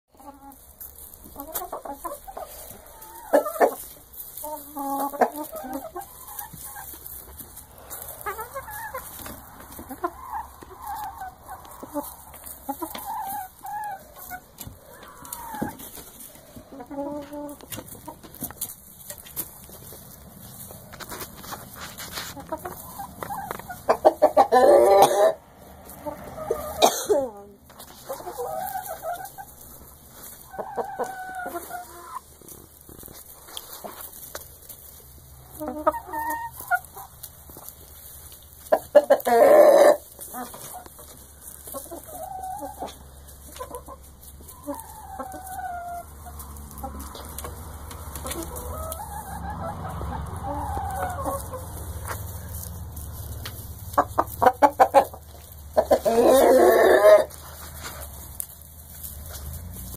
My chickens
These are some of my chickens I also have 20 smaller chickens